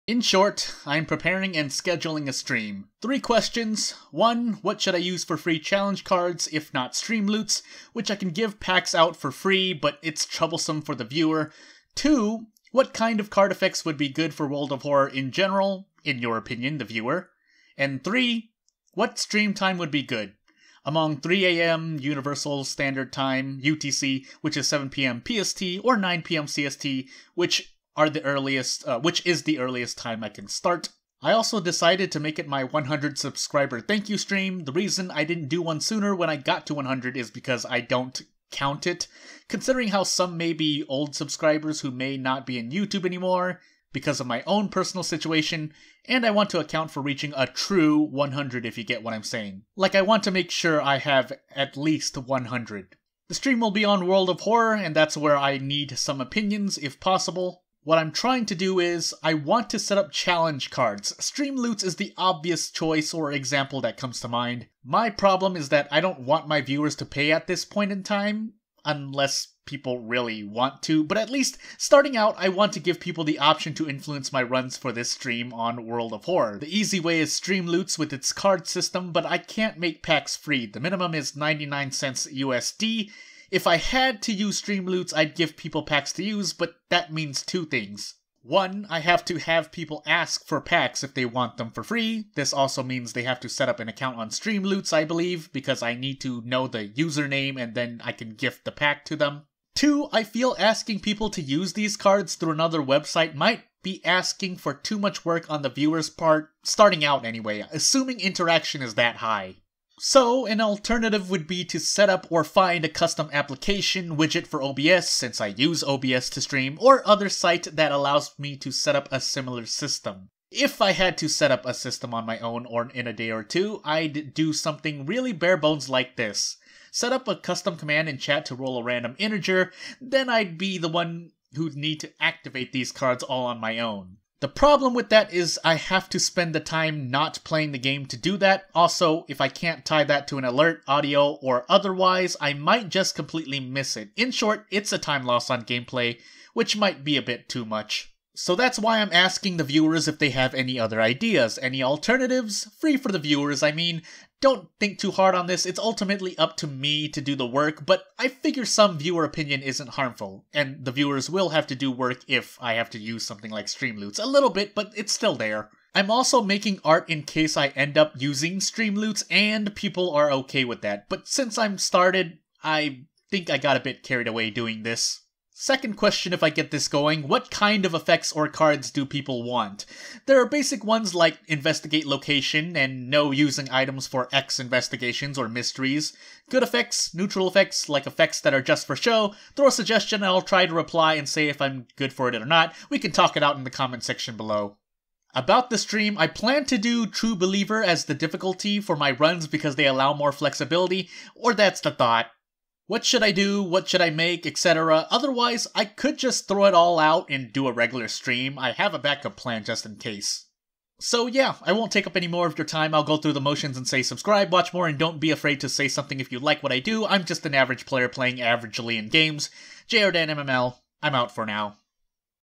Asking For Opinions From Viewers - Stream Preparations
[Stream] Preparing a World of Horror stream + Thanks for 100 subs! And I want to make the stream spicier. Maybe I should make a Discord channel...